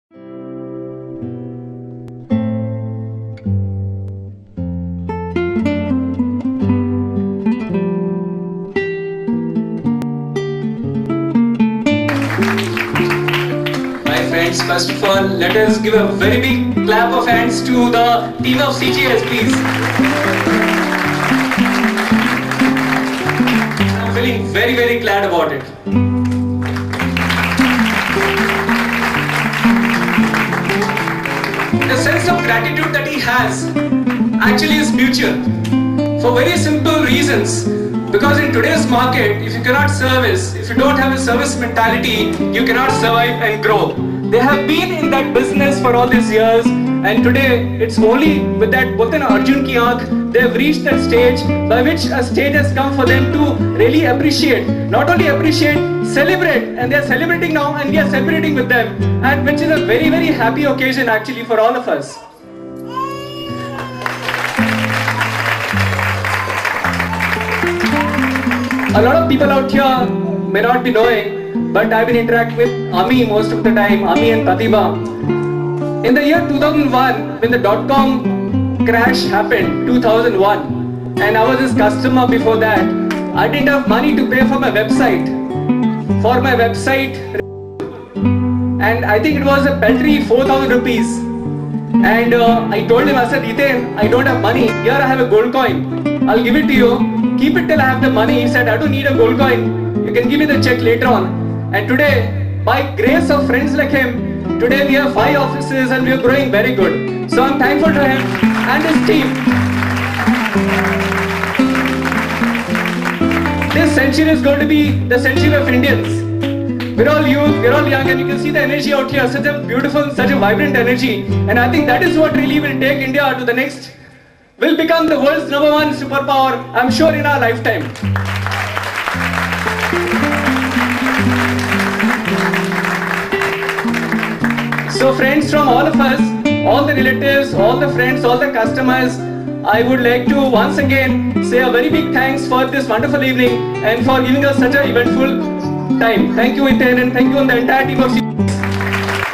search engine optimization,website design,SEO
This is a testimonial from Mr. Pankaj a honorable CEO of HR Mantra, Mumbai. He is very satisfy with the work done by CGS infotech for getting his website top in yahoo, google,aol and other search engine. He has describe the dedication and integrity  for the work of CGS team members.